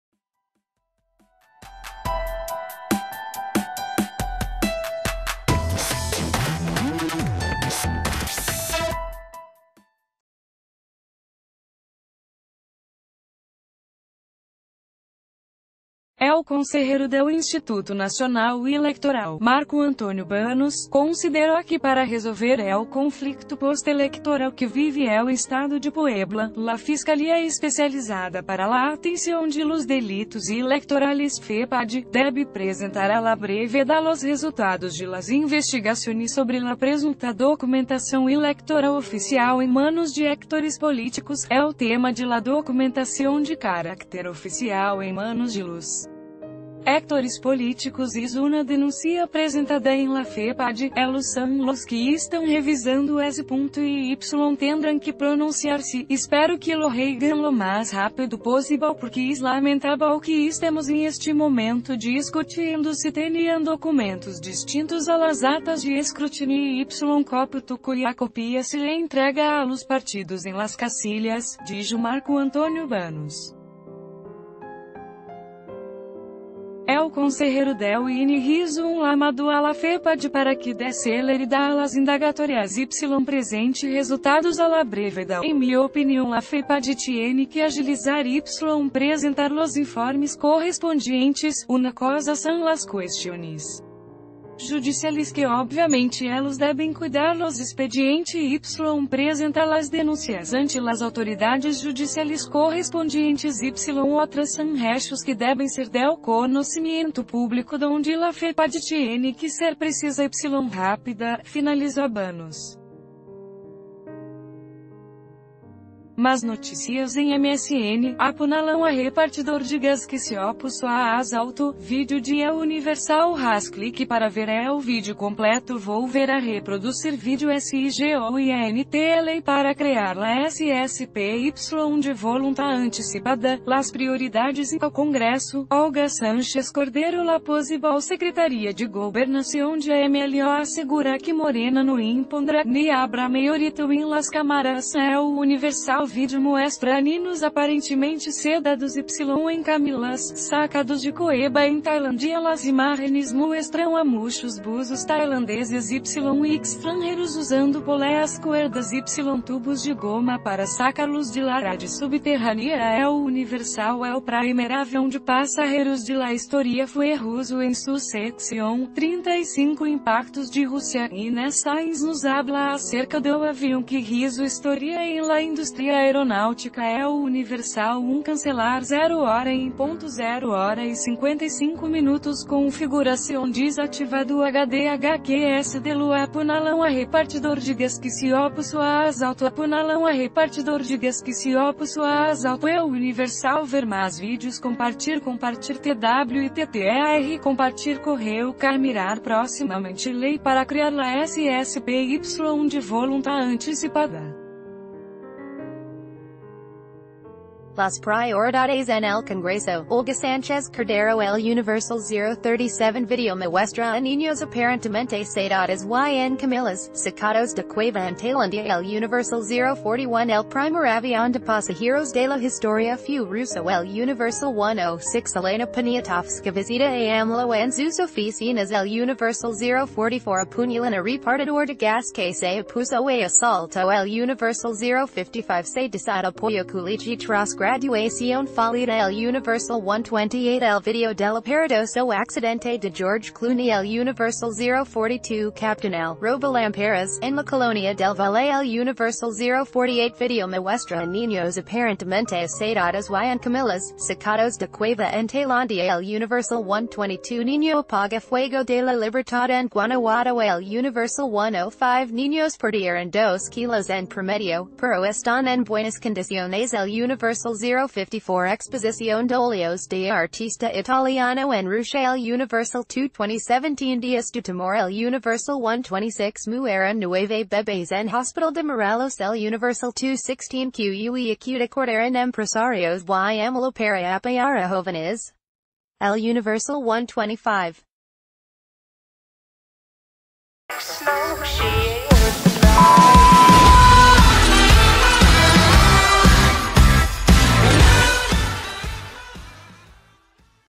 INE pide a Fepade investigar irregularidades en elección de Puebla
El consejero del Instituto Nacional Electoral, Marco Antonio Baños, consideró que para resolver el conflicto postelectoral que vive el estado de Puebla, la Fiscalía Especializada para la Atención de los Delitos Electorales (Fepade), debe presentar a la brevedad los resultados de las investigaciones ...